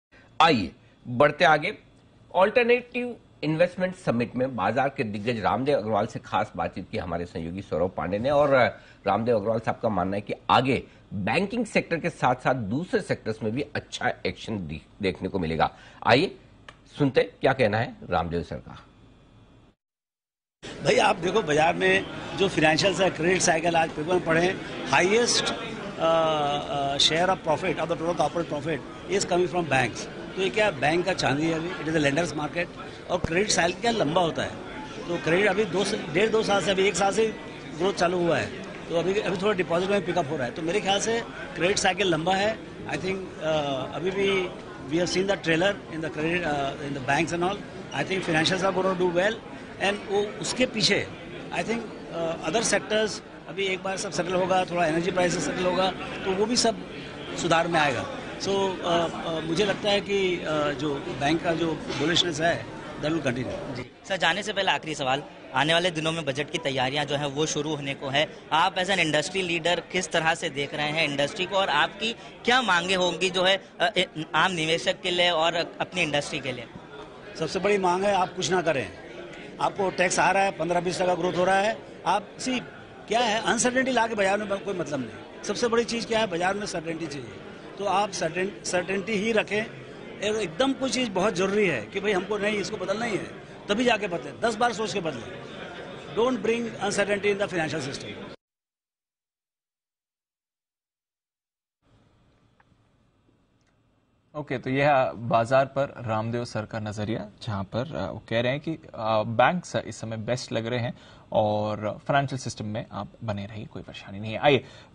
Budget Expectation: Market's expectation from Budget 2023 | From Chairman of MOFSL Raamdeo Agrawal
किन सेक्टर में आगे दिखेगा एक्शन❓

📊Budget को लेकर क्या है बाजार की मांग?

देखिए MOFSL के चेयरमैन रामदेव अग्रवाल के साथ खास बातचीत